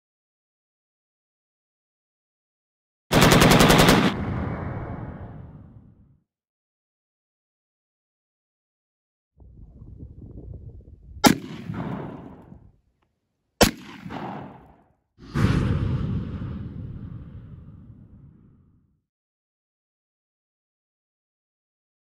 Remington 870 Marine Magnum Shooting Water Jugs
Camo’s 2A USA

-The Gun
Remington 870 Marine Magnum 12 Gauge

-The Ammo
Black Aces 00 Buck

Camo’s 2A USA